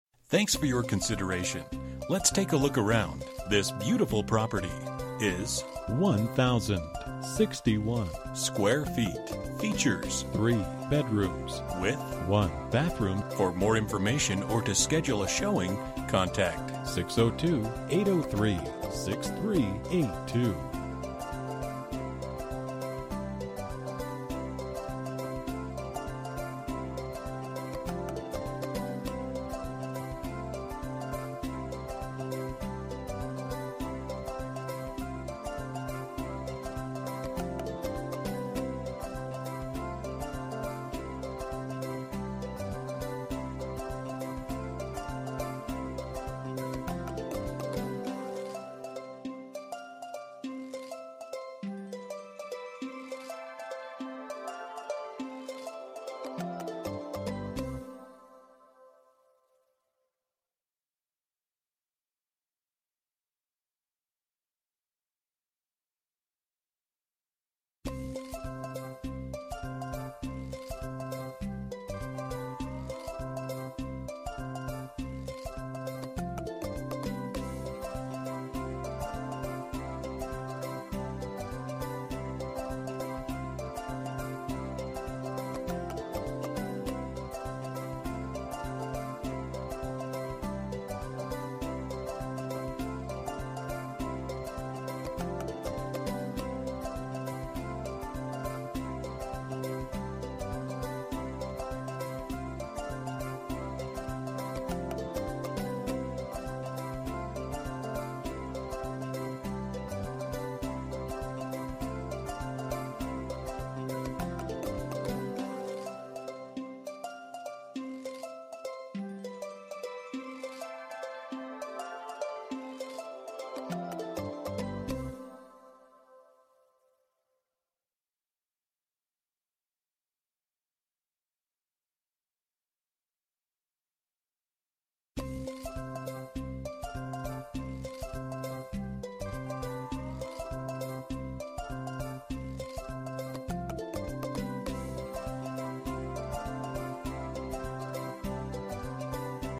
Residential for sale - 1628 W MICHELLE Drive, Phoenix, AZ 85023
Hurry and see this nice ranch style home in a quiet neighborhood.  Upgraded features and new interior paint. Large backyard with RV gate and fenced RV or trailer parking. Large storage shed.  High quality appliances and home warranty included.  Get your clients in to see this quick because it won't last long!

Bedrooms: 3
Bathrooms: 1 Bathroom
Square Feet: 1061
Price: $164,000

For more information about this property, please contact Mark David Womack at or . You can also text 3521643 to 67299.